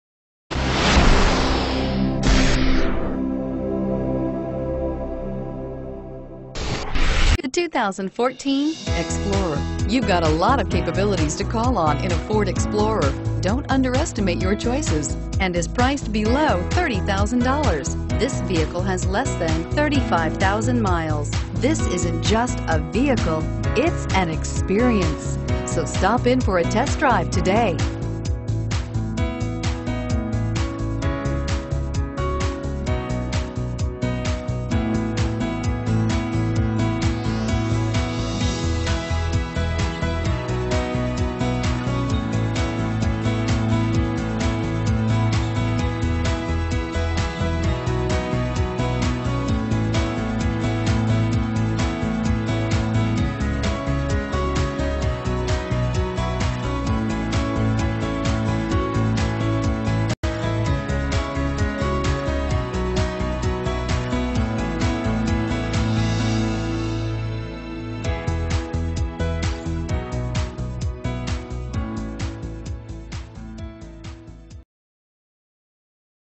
2014 Ford Explorer Dallas Ft. Worth Grapevine GEGC35174
Grubbs Infiniti
1500 E State Highway 114

* 3.5 LITER 6 Cylinder engine * * ONLY ONE PREVIOUS OWNER * * Anti-lock Brakes ** A Backup Camera ** Parking Assistance ** Traction Control ** Side Air Bag System * * 2014 ** Ford * * Explorer * * XLT * It has a ruby red metallic tinted clearcoat exterior and a charcoal black interior. With an unbeatable 5-star crash test rating, this SUV puts safety first. According to a review from CarsDirect, Performance in government crash tests is good. Looking for a specific model? Call today for more information.